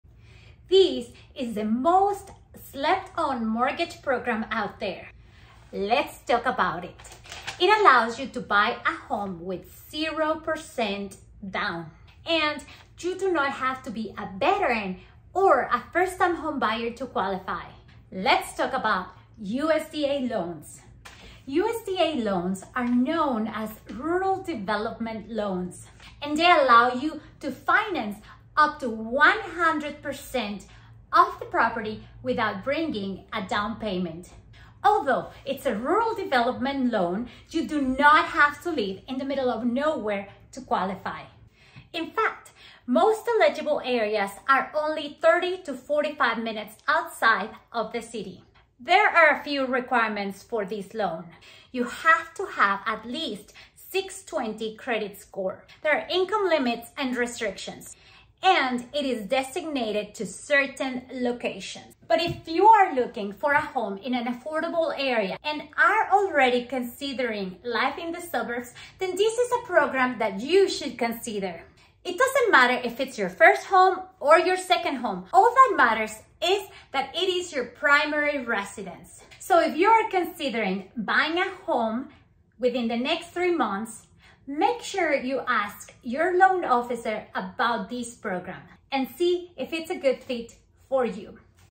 BUY a HOUSE with 0% Down
And this is NOT for Fist Time Home Buyers only!

USDA loans are zero down payment mortgages that can open the path for suburban buyers!

The requirements for these loans are based on location and income.

If you are looking to buy a home in the suburbs, let’s talk!